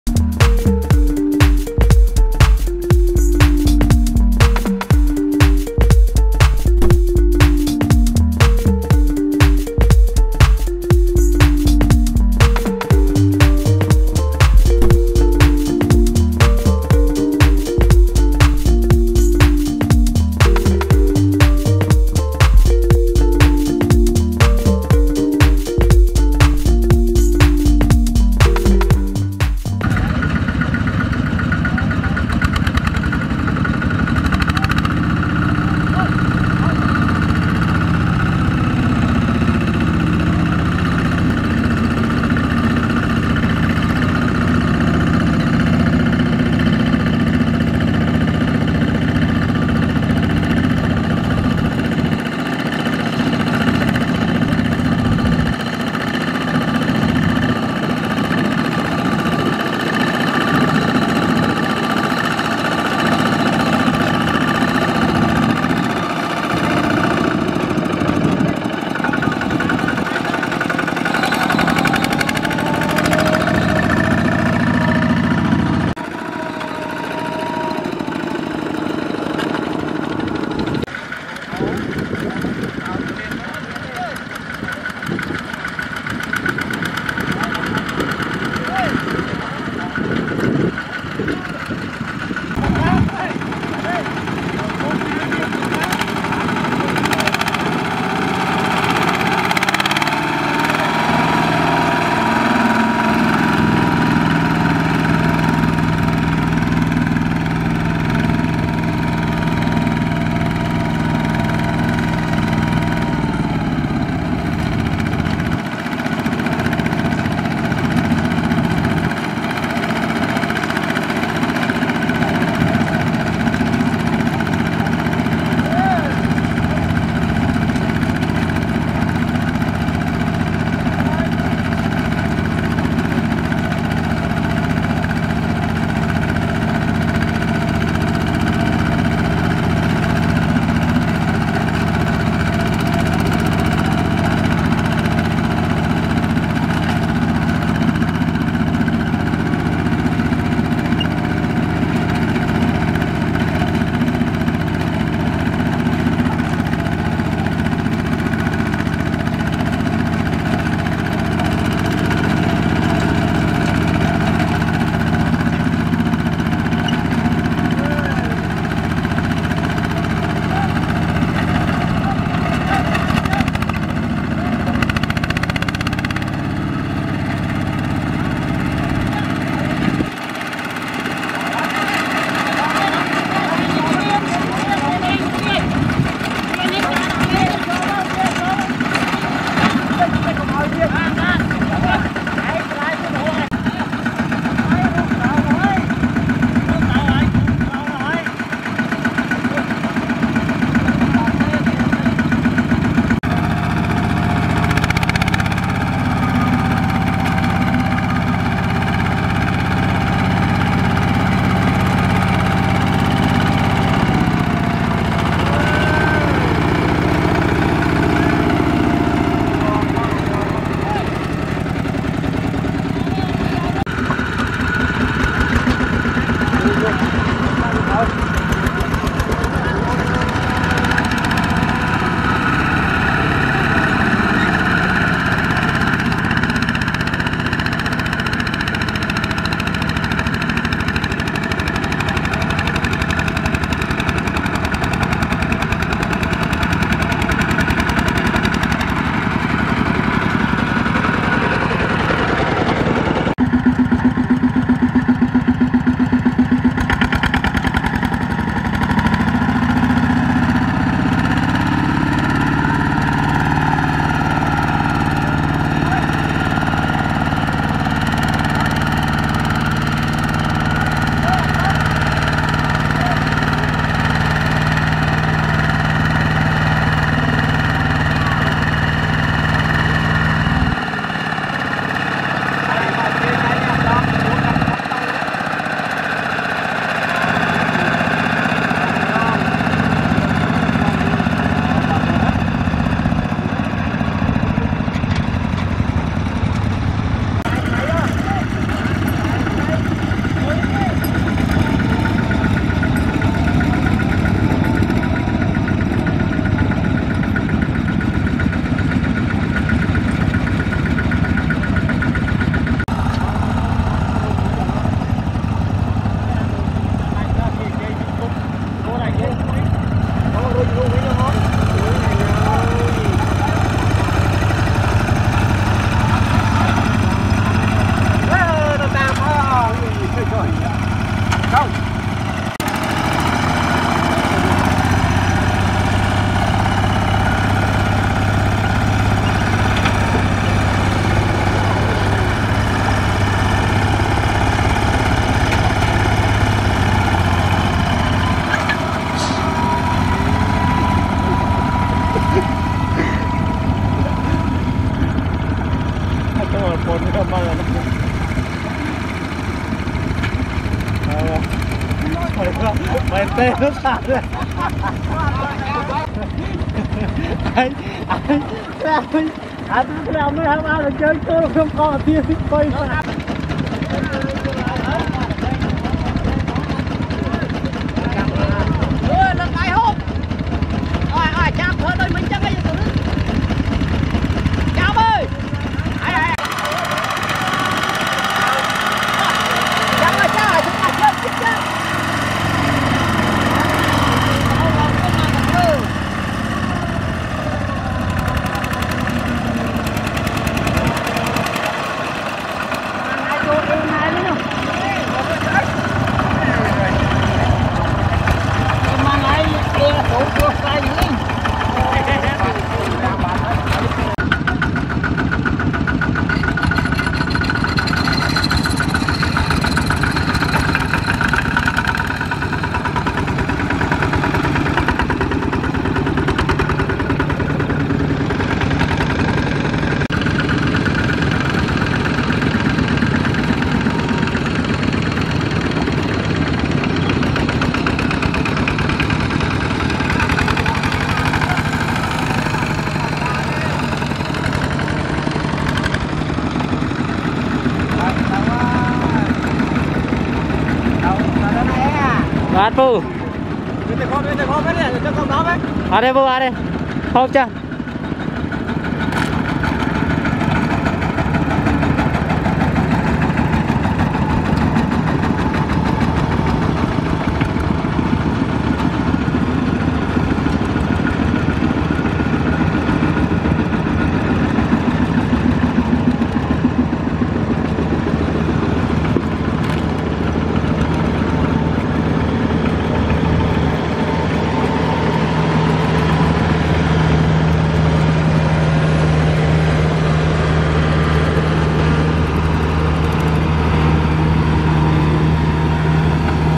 Tractor Kubota ZT120 jump to high dam so dangerous / គោយន្តដឹកស្រូវច្រើនឡើងទំនប់ខ្ពស់ខាវដីល្អមើលណាស់
ឆាណែលរបស់យើងខ្ញុំបង្កើតឡើងដើម្បីបង្ហាញអោយអ្នកទាំងអស់គ្នាបានដឹងពីការរីកចម្រើនផ្នែកខាងវិស័យកសិកម្មនៅកម្ពុជា។
ការប្រមូលផលស្រូវរបស់ប្រជាកសិករនៅ​​​ ស្រុក គិរីវង់ ខេត្ត​ តាកែវ។